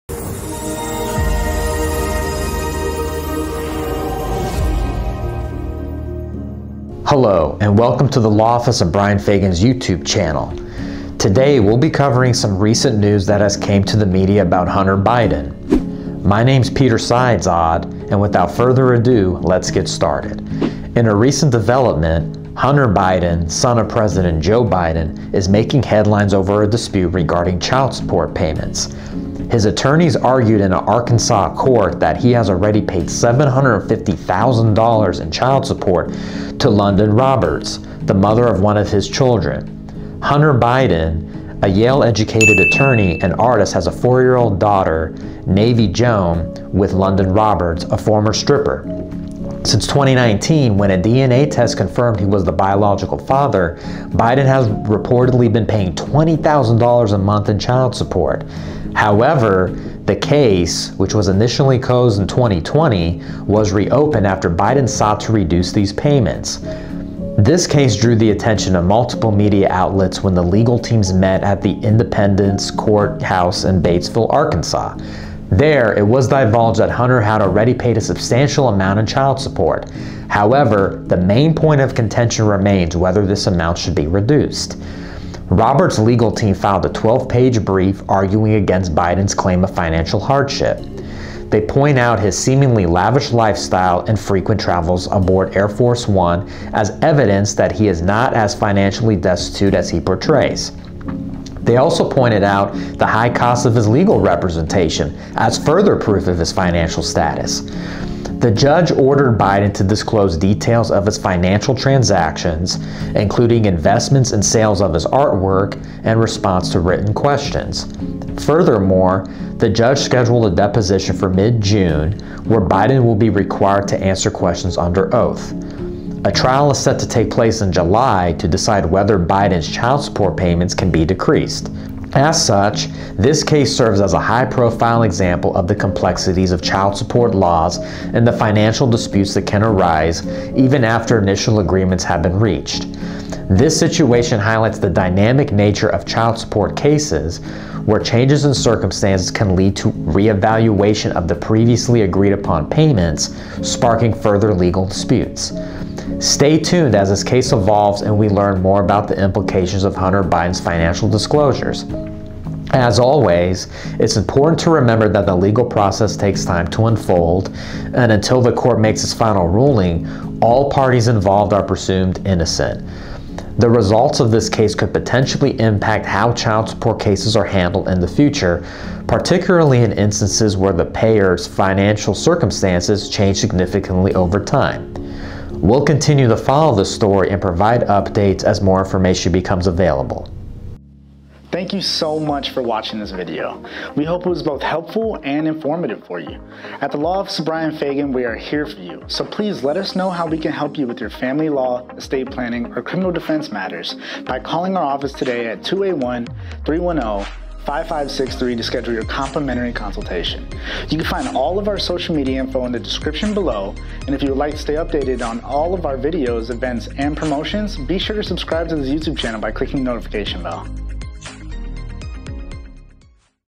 Inside Look: The Hunter Biden Child Support Case - Unveiling The Facts
Welcome to our deep-dive analysis of the widely talked about Hunter Biden child support case. We've sifted through the noise to bring you a comprehensive, unbiased breakdown of the proceedings, their impacts, and what they might mean for the Biden family and beyond.

In this video, we explore the intricate timeline of the case, analyzing critical court documents and legal jargon to provide you with a clear understanding of what transpired. We've also enlisted the expert opinion of a legal attorney to shed light on the nuances of the case.